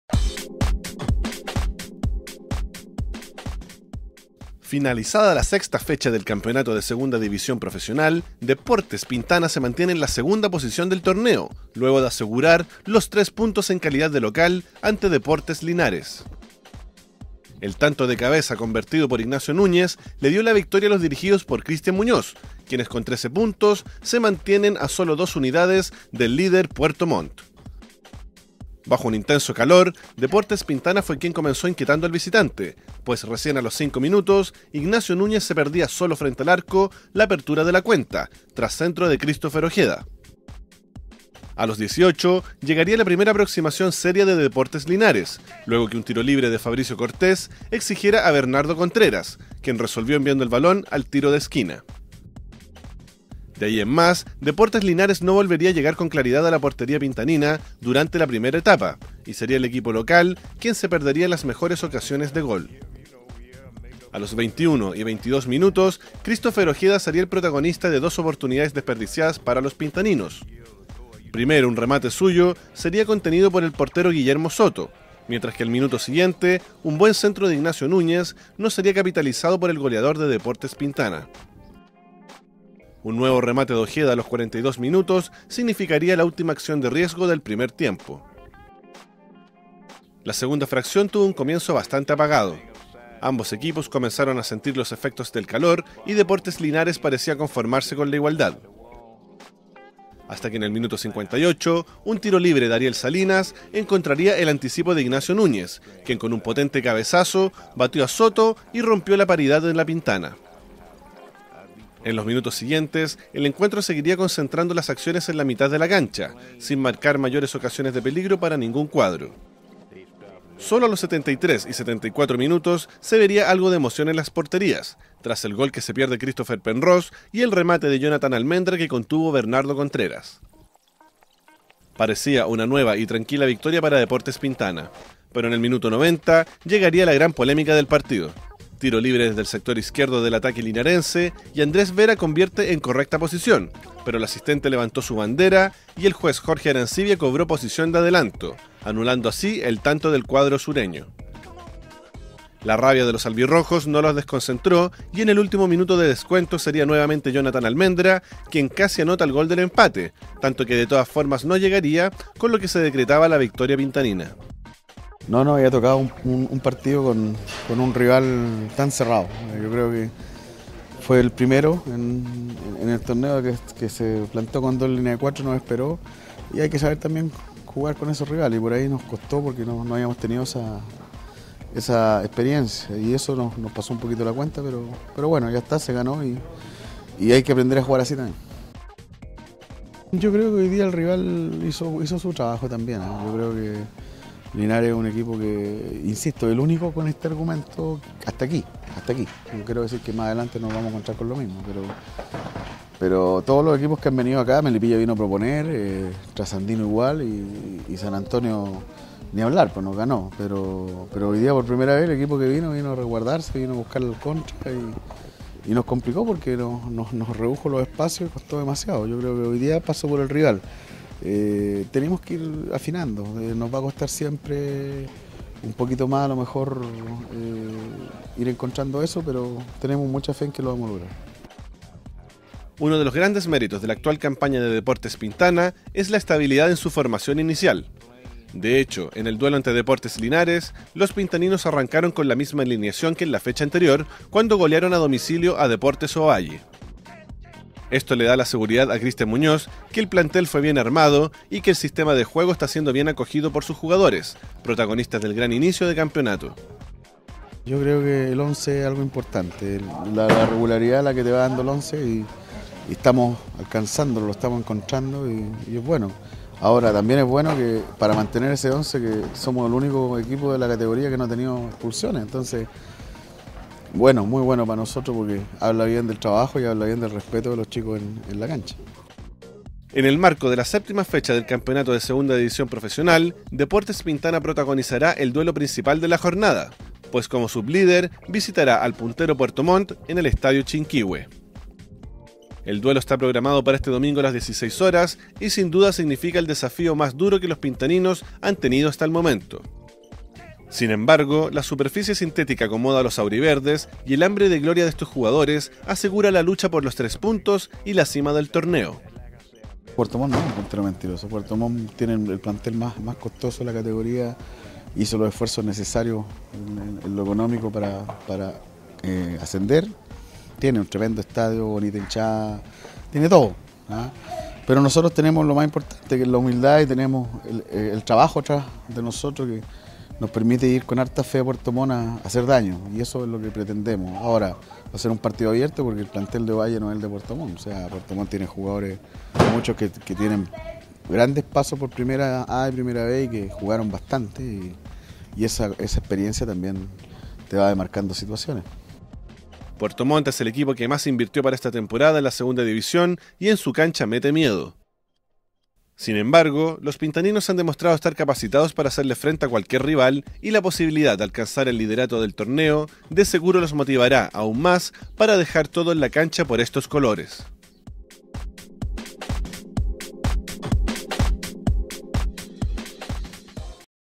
Deportes Pintana vence a Linares y ahora va por el puntero
Deportes Pintana sigue a la caza de Puerto Montt en el Campeonato de Segunda División. Tras vencer por 1 gol a cero a Deportes Linares, los pintaninos se mantienen a dos puntos del cuadro “salmonero”, a quienes visitarán este sábado con la ilusión de alcanzar la punta del torneo.